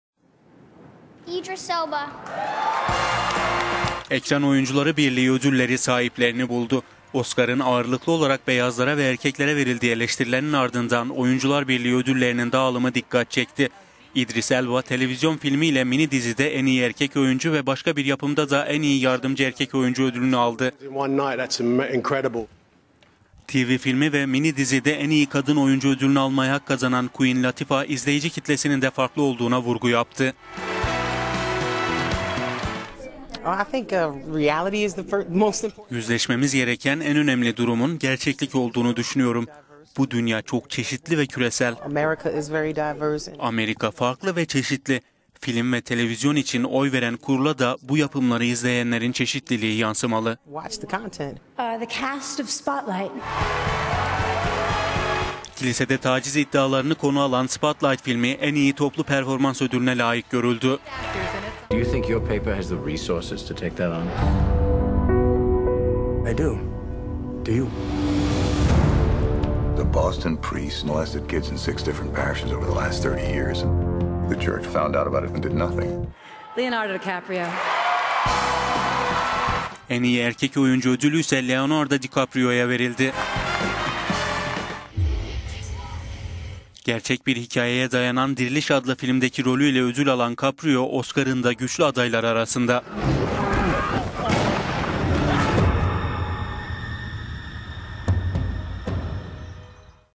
Ekran Oyuncuları Birliği Ödülleri'ne 'farklılık' damgası
Oscar'ın habercisi olarak görülen Ekran Oyuncuları Birliği Ödülleri sahiplerini buldu.

Oscar'ın ağırlıklı olarak beyazlara ve erkeklere  verildiği eleştirilerinin ardından Oyuncular Birliği Ödülleri'nin dağılımı dikkat çekti. Gecede siyah ve kadın oyuncular da ödüle layık görüldü.

Idris Elba, televizyon filmi ile mini dizide "en iyi erkek oyuncu" ve başka bir yapımda "en iyi yardımcı erkek oyuncu" seçilerek iki ödül birden aldı.

Televizyon filmi ve mini dizide "en iyi kadın oyuncu" ödülün…